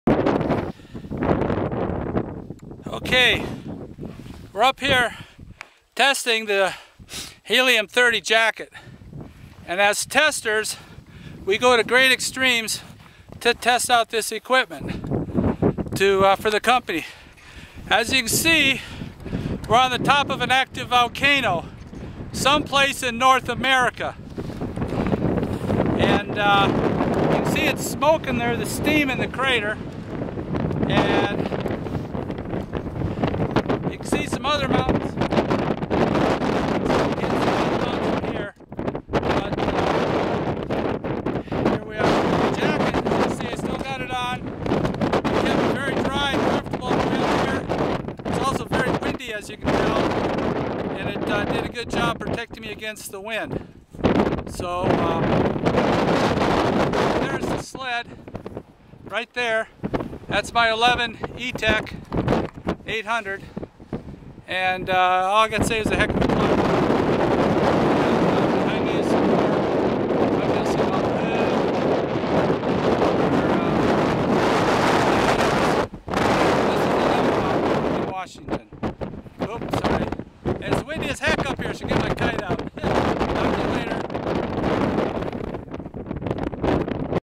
Volcano Testing the Helium 30 Jacket
Testing the Ski-Doo Helium 30 Jacket on the top of an active volcano.

This is uncut raw footage.